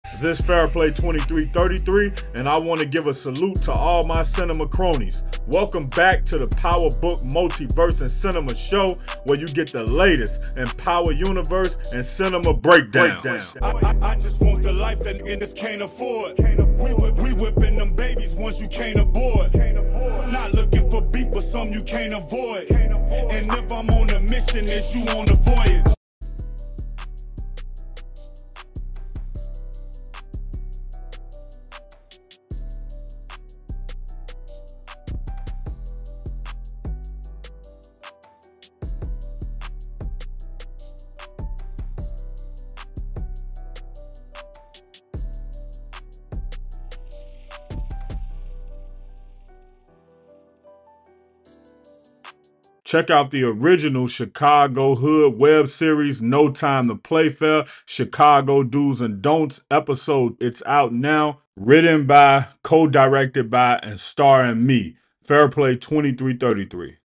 Is Episode Leak Real? BLANCA RODRIGUEZ Killed, Power Book II Ghost Season 4
Power Leak! BLANCA RODRIGUEZ Killed, Power Book II Ghost Season 4

$SSDF312

DONATE to the growth of the channel as I film independent movies, short, films, and drop none stop content.

GHOST is Alive! Power Book 1: James St Patrick Theories